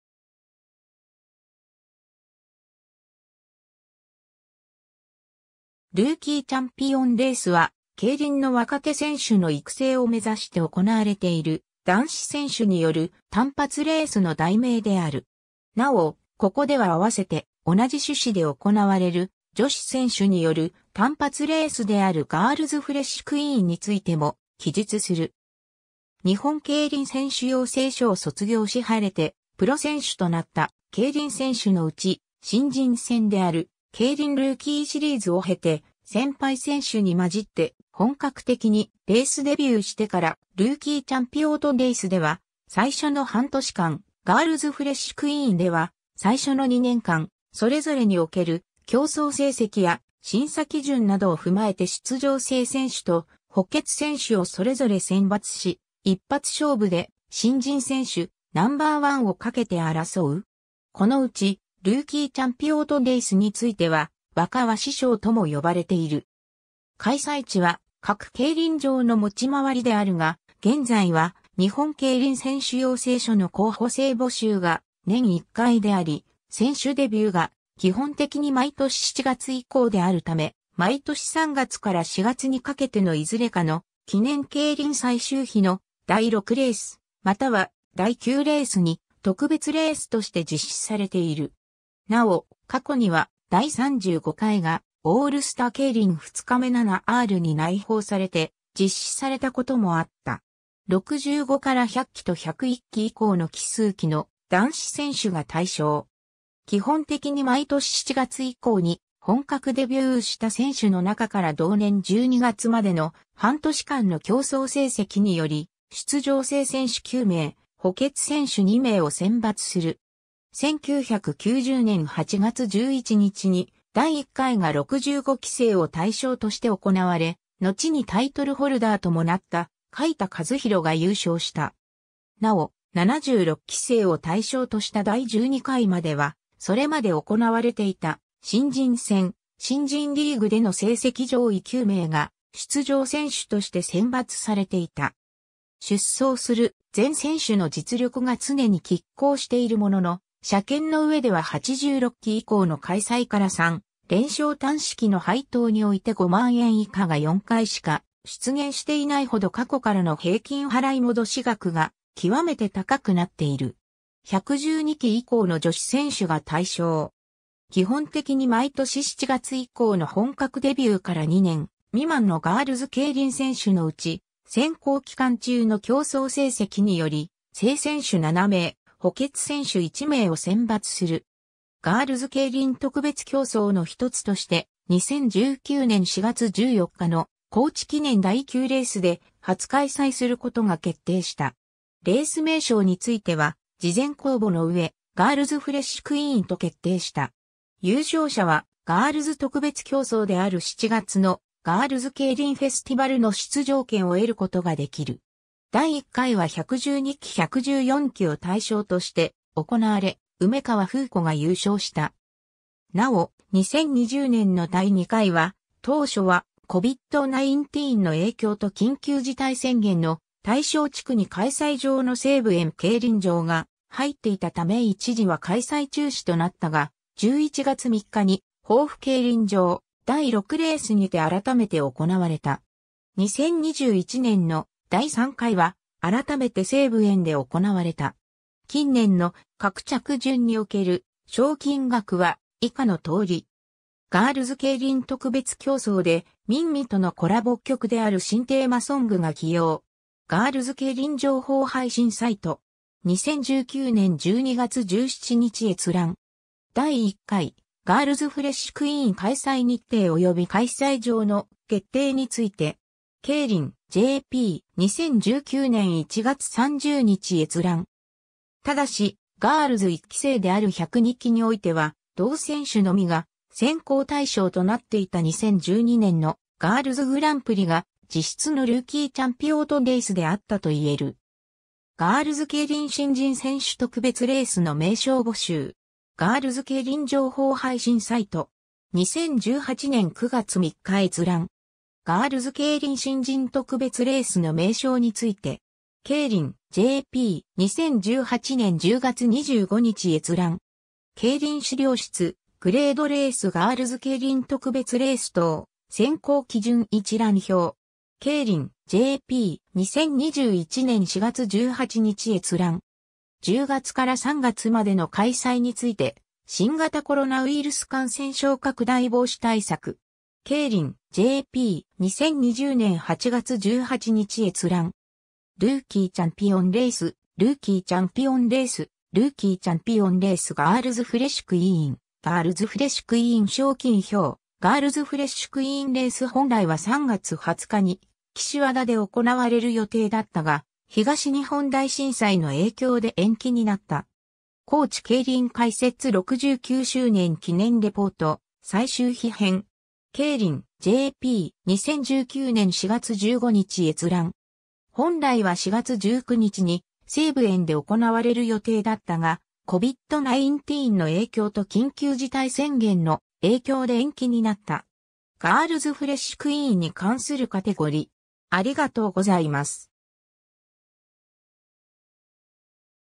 ルーキーチャンピオンレース
ルーキーチャンピオンレース（NC。通称・若鷲賞）は、競輪の若手選手の育成を目指して行われている、男子選手による単発レースの題名である。
 なお、ここでは併せて、同じ趣旨で行われる、女子選手による単発レースであるガールズ フレッシュクイーン（GFQ）についても記述する。
 日本競輪選手養成所を卒業し晴れてプロ選手となった競輪選手のうち、新人戦である「競輪ルーキーシリーズ」を経て（養成所早期卒業者を除く）、先輩選手に混じって本格的にレースデビューしてから、「ルーキーチャンピオンレース」（男子）では最初の半年間、「ガールズ フレッシュクイーン」（女子）では最初の2年間、それぞれにおける競走成績や審査基準などを踏まえて出場正選手と補欠選手をそれぞれ選抜し、一発勝負で新人選手ナンバーワンをかけて争う。
このうち、「ルーキーチャンピオンレース」については『若鷲賞』とも呼ばれている。
 開催地は各競輪場の持ち回りであるが、現在は日本競輪選手養成所の候補生募集が年1回であり選手デビューが基本的に毎年7月以降であるため、毎年3月から4月にかけてのいずれかの記念競輪（GIII）最終日（4日目）の第6レースまたは第9レースに、特別レース（企画レース）として実施されている。
なお、過去には第35回がオールスター競輪（GI）2日目7Rに内包されて実施されたこともあった。
 65 - 100期と101期以降の奇数期の男子選手が対象。
基本的に毎年7月以降に本格デビューした選手の中から同年12月までの半年間の競走成績により、出場正選手9名・補欠選手2名を選抜する。
1990年8月11日に第1回が65期生を対象として行われ、のちにタイトルホルダーともなった海田和裕が優勝した。
なお、76期生を対象とした第12回までは、それまで行われていた新人戦「新人リーグ」での成績上位9名が出場選手として選抜されていた。
出走する全選手の実力が常に拮抗しているものの、車券の上では86期以降の開催から三連勝単式の配当において5万円以下が4回しか出現していないほど過去からの平均払戻額が極めて高くなっている。
 112期以降の女子選手（いずれも偶数期）が対象。
基本的に毎年7月以降の本格デビューから2年未満のガールズケイリン選手のうち、選考期間（毎年7月 - 12月）中の競走成績により、正選手7名・補欠選手1名を選抜する。
ガールズケイリン特別競走の一つとして、2019年4月14日の高知記念（GIII）第9レースで初開催することが決定した。
レース名称については事前公募の上、「ガールズ フレッシュクイーン」と決定した。
優勝者は、ガールズ特別競走である7月のガールズケイリンフェスティバルの出場権を得ることができる。
第1回は112期・114期を対象として行われ、梅川風子が優勝した。
なお、2020年の第2回は、当初はCOVID-19の影響と緊急事態宣言の対象地区に開催場の西武園競輪場が入っていたため一時は開催中止となったが、11月3日に防府競輪場第6レースにて改めて行われた。
2021年の第3回は、改めて西武園で行われた。
 近年の各着順における賞金額は、以下の通り。
 ^ “ガールズケイリン特別競走でMINMIとのコラボ曲である新テーマソングが起用！”. ガールズケイリン情報配信サイト (2019年12月17日). 2019年12月17日閲覧。
 ^ “第1回ガールズ　フレッシュクイーン開催日程及び開催場の決定について”. KEIRIN.JP (2019年1月30日). 2019年1月30日閲覧。
 ^ 但し、ガールズ1期生である102期においては、同選手のみが選考対象となっていた2012年のガールズグランプリが実質のルーキーチャンピオンレースであったと言える。
 ^ “ガールズケイリン新人選手特別レースの名称募集！”. ガールズケイリン情報配信サイト (2018年9月3日). 2018年9月3日閲覧。
 ^ “ガールズケイリン新人特別レースの名称について”. KEIRIN.JP (2018年10月25日). 2018年10月25日閲覧。
 ^ “競輪資料室　グレードレース・ガールズケイリン特別レース等選考基準一覧表（「ガールズケイリンフェスティバル」の項目をクリック）”. KEIRIN.JP. 2021年4月18日閲覧。
 ^ “10月から3月までの開催について～新型コロナウイルス感染症拡大防止対策～”. KEIRIN.JP (2020年8月17日). 2...